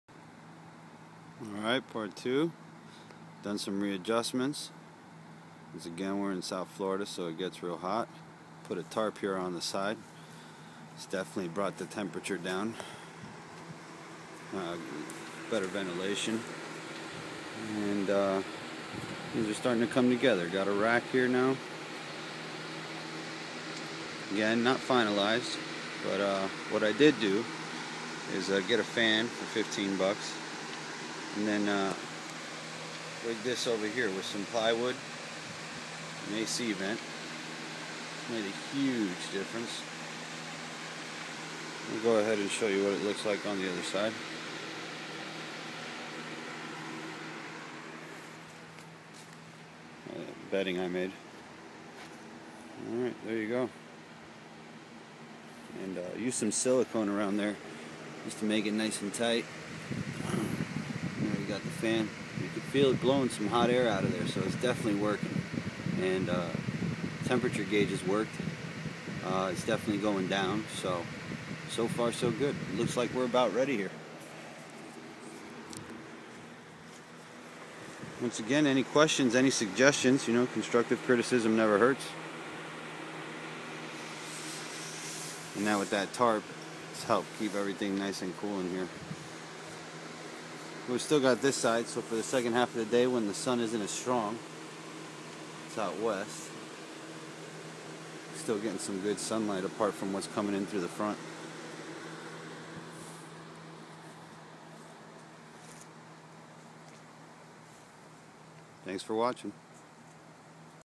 PVC Dome Greenhouse and Hydroponics Pt. 2
Added more ventilation and shade...